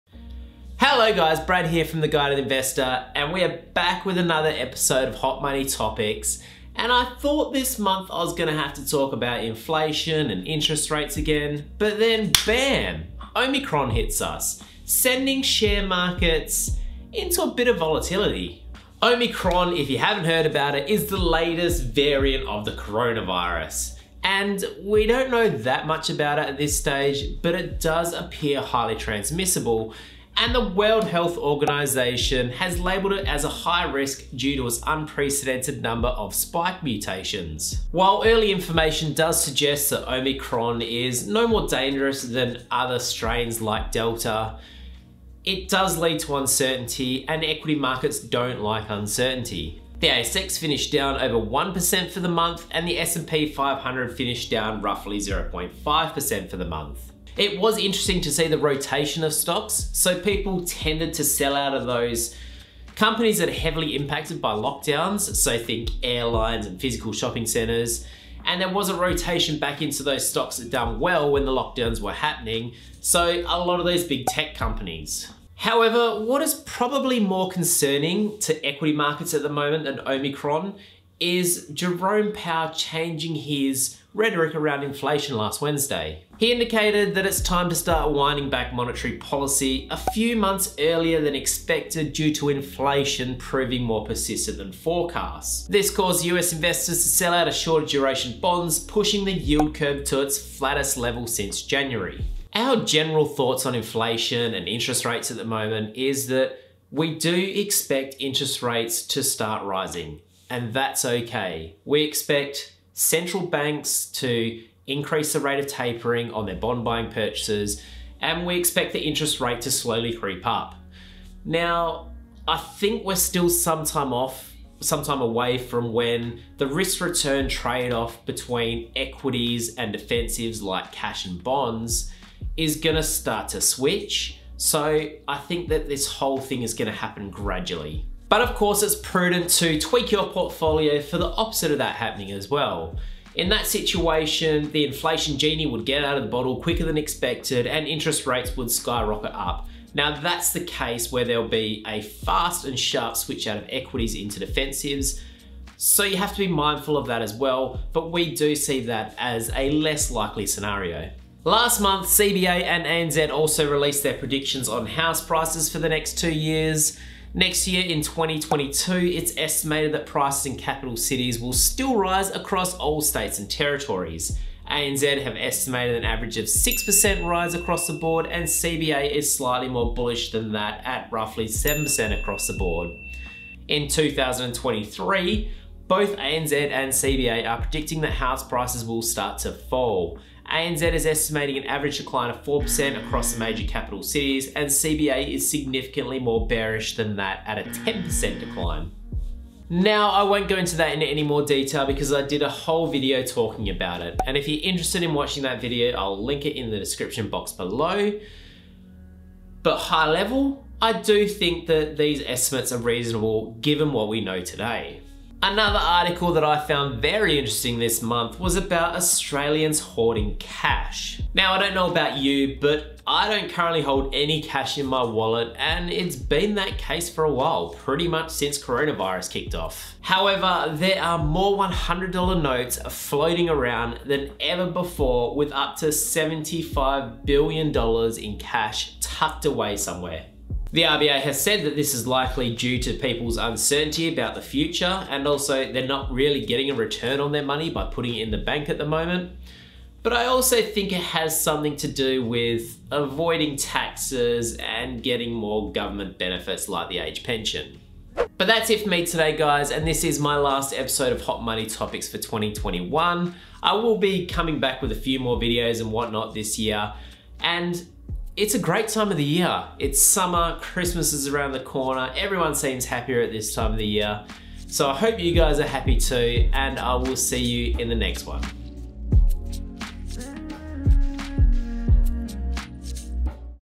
Omicron causes volatility | Powell talks inflation | Property predicted to drop
In this episode of Hot Money Topics we talk about Omicron causing volatility in share markets, Jerome Powell changing his rhetoric on inflation, property prices predicted to drop by two of Australia's largest banks and more physical $100 noted in the economy than ever before.